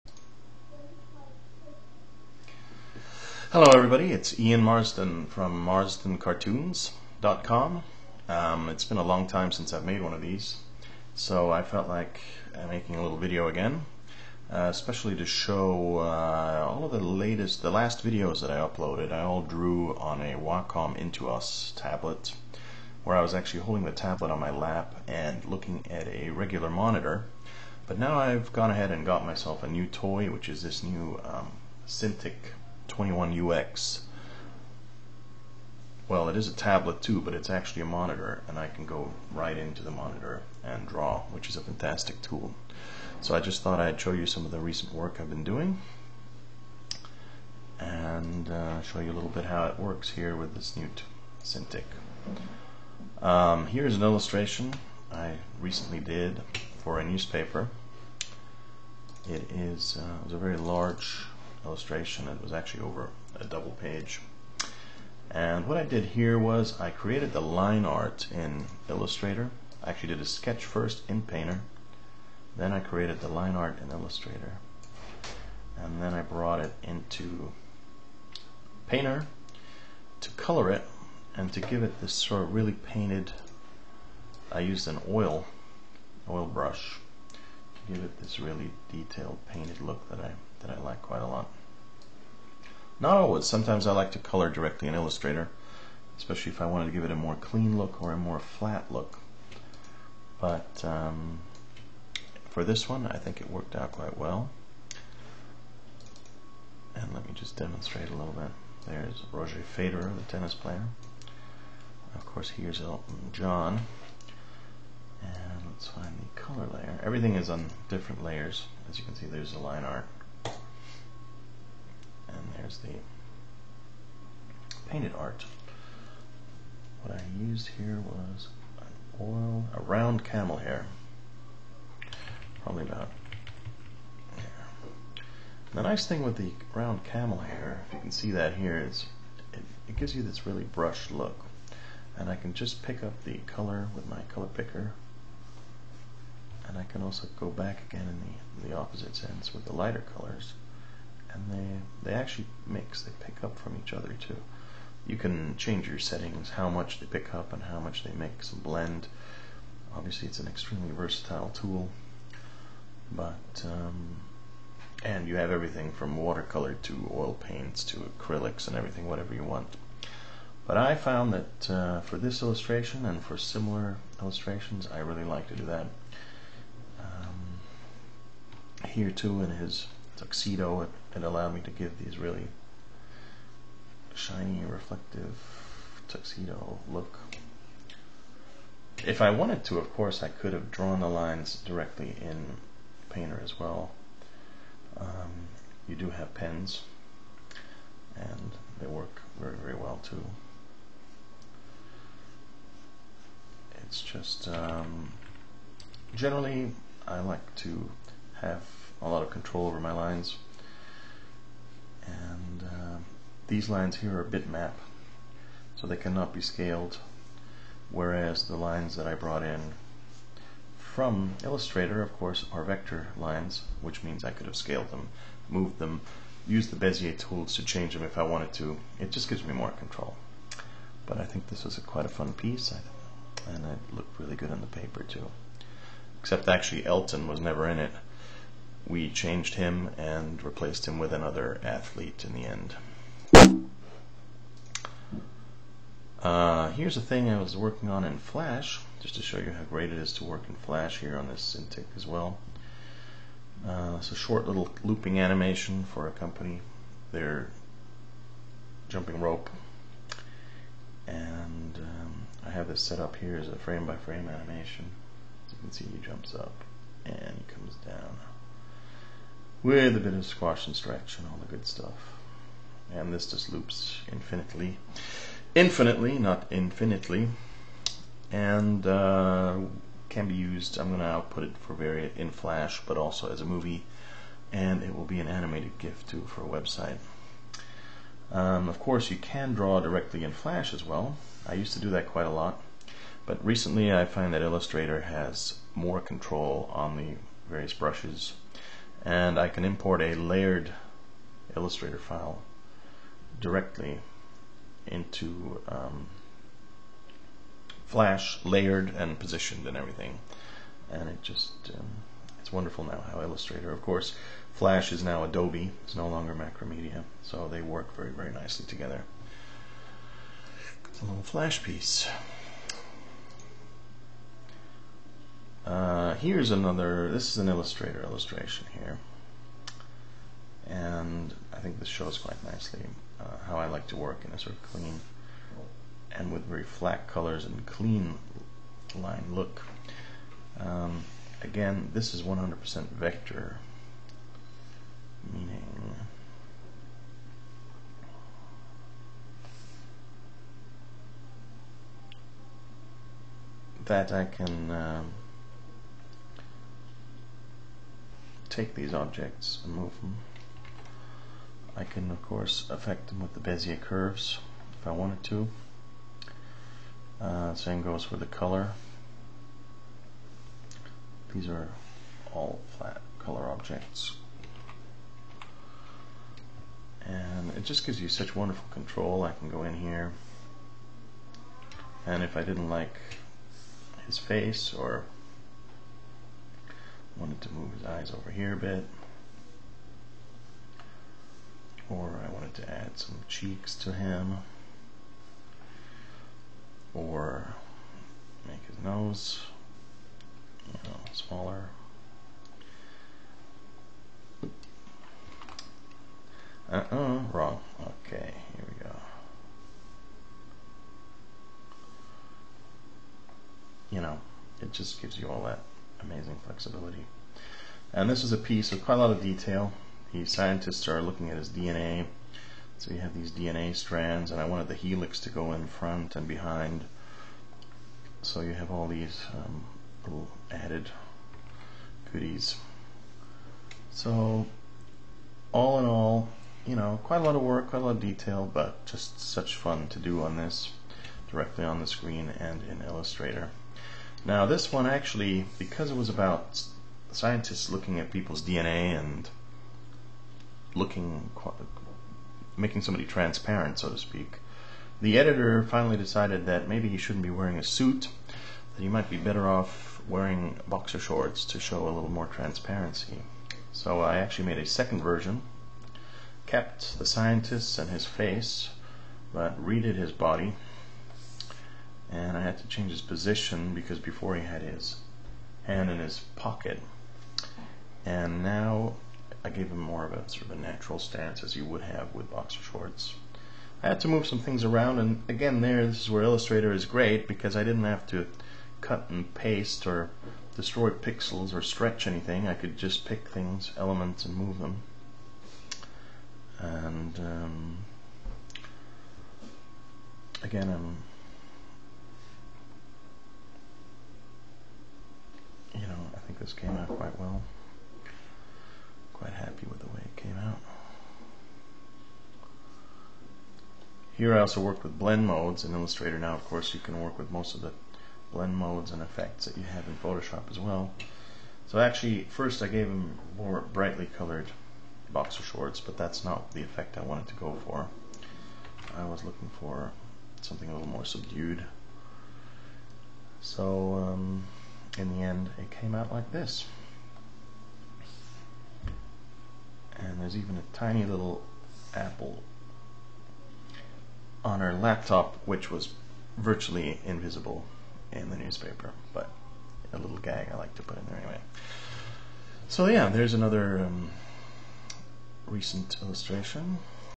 Behind the Scenes: Illustrations on Wacom Cintiq 21UX Tablet
Illustrations on Wacom Cintiq 21UX Tablet

I am a professional illustrator and internationally published cartoonist and graphic novel author and artist with 20+ years experience.

Feel free to pose questions in the comments, I will try to answer as efficiently as possible.

Enjoy!  Ian